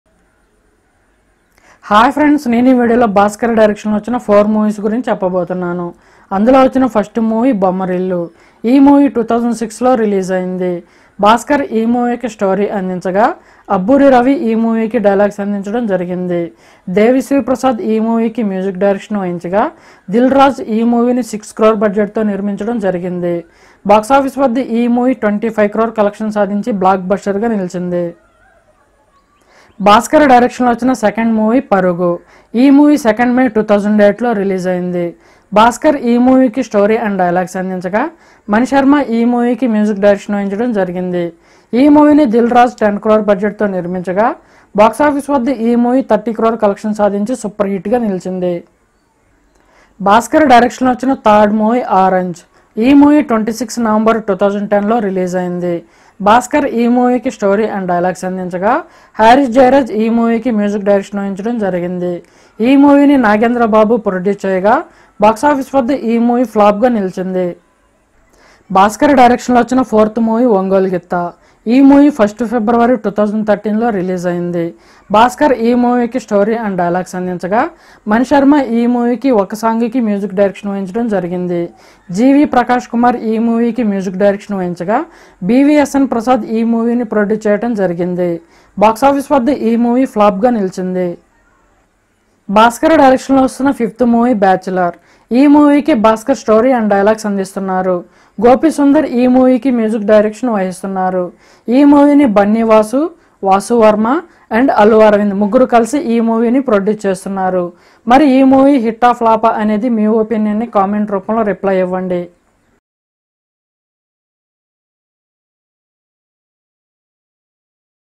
Tollywood Heroes Movies in Bommarillu BHASKAR Direction|BHASKAR Hits and Flops Movies List in Telugu
Bhaskar is a Director in Tollywood. He Directed Bommarillu Movie Starring Siddardah, Parugu Movie Starring Allu Arjun, Orange Movie Starring Ram Charan, Ongole Gittha Movie Starring Ram Pothineni and Upcoming Movie Bachelor Movie Starring Akhil and Pooja Hegde.